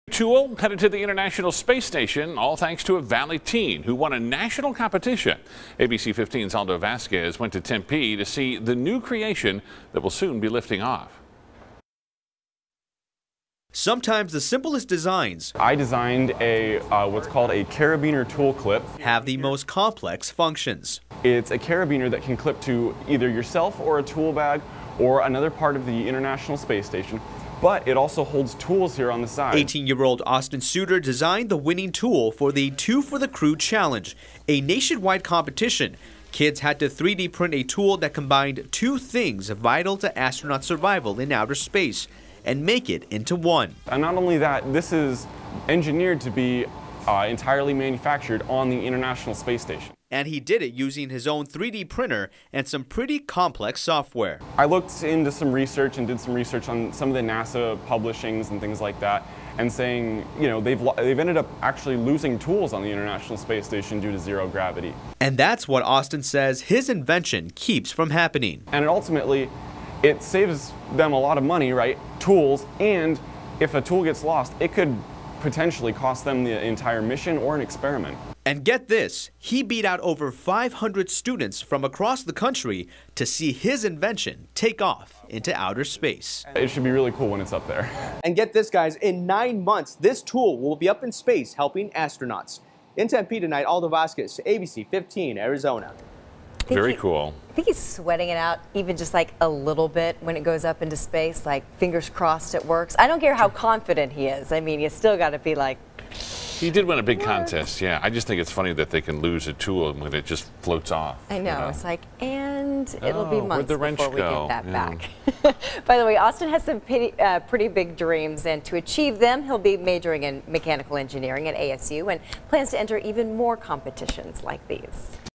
Valley teen's tool picked for use on International Space Station
NASA held a contest challenging students to develop a tool that had to be 3D printed and combine the functionality of at least two tools.